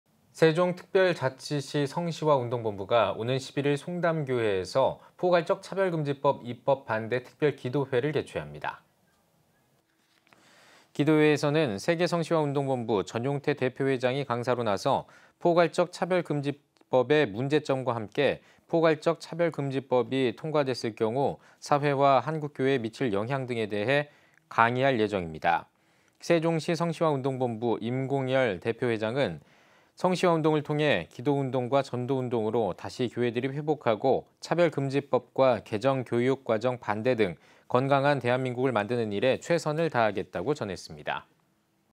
세종특별자치시성시화운동본부, 포괄적 차별금지법 입법 반대 특별 기도회 개최 (이한승) l CTS뉴스
세종특별자치시성시화운동본부가 오는 11일 송담교회에서 ‘포괄적 차별금지법 입법 반대 특별 기도회’를 개최합니다.

기도회에서는 세계성시화운동본부 전용태 대표회장이 강사로 나서 포괄적 차별금지법의 문제점과 함께 포괄적 차별금지법이 통과됐을 경우 사회와 한국교회에 미칠 영향 등에 관해 강의할 예정입니다.

세종시성시화운동본부 임공열 대표회장은 “성시화운동을 통해 기도 운동과 전도 운동으로 다시 교회들이 회복하고 차별금지법과 개정 교육과정 반대 등 건강한 대한민국을 만드는 일에 최선을 다하겠다”고 전했습니다.